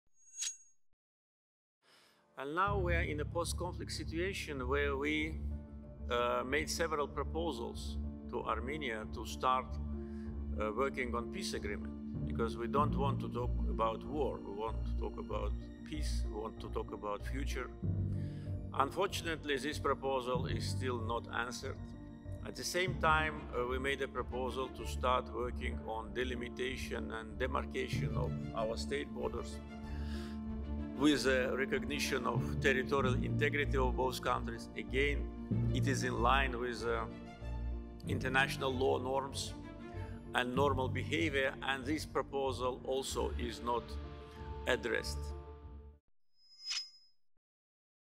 Biz Ermənistana bir sıra təkliflər etmişik ki, sülh müqaviləsi üzərində işləməyə başlayaq.
İlham Əliyev: “İndi biz postmünaqişə vəziyyətindəyik və biz Ermənistana bir sıra təkliflər etmişik ki, sülh müqaviləsi üzərində işləməyə başlayaq, çünki biz müharibə deyil, sülh, gələcək haqqında danışmaq istəyirik. Əfsuslar olsun ki, bu təklifə hələ də cavab verilməyib”.